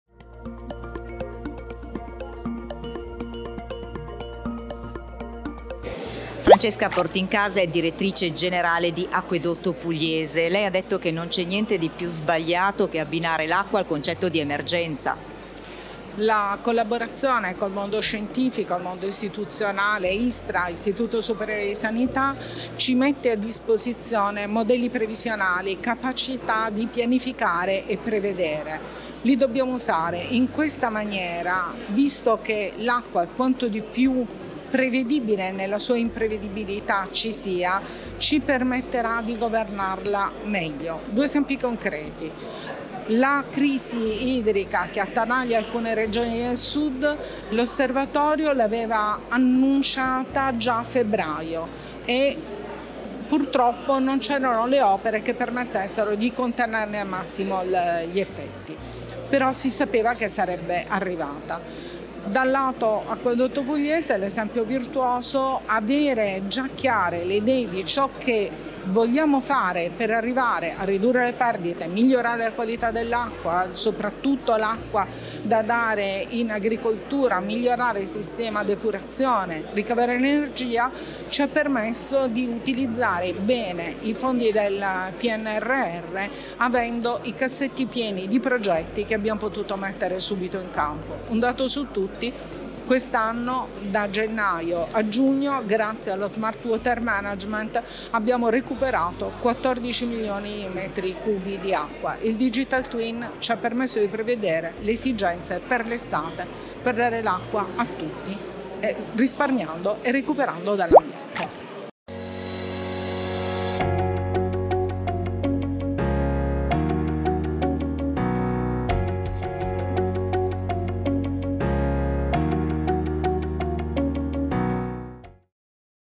Festival dell'Acqua 2024 - Francesca Portincasa - Acquedotto Pugliese
ACQUA, FIRENZE OSPITA L’8A EDIZIONE DEL FESTIVAL DI UTILITALIA.
Dal 24 al 26 settembre Firenze ospita il Festival dell’Acqua 2024, uno fra i principali appuntamenti nazionali incentrati sui temi del servizio idrico.
Ideato e promosso da Utilitalia, il Festival dell’Acqua è giunto all’ottava edizione e nel 2024 è organizzato in collaborazione con Publiacqua e Confservizi Cispel Toscana